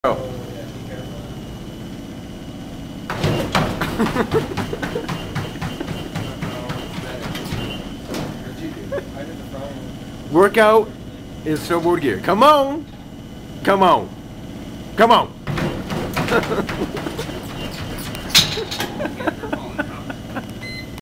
Roon Treadmill Running With Snowboard Gear
Roon goes for a couple sprints with his "Vocalize Silence" R&B gear.  Ain't no sweat.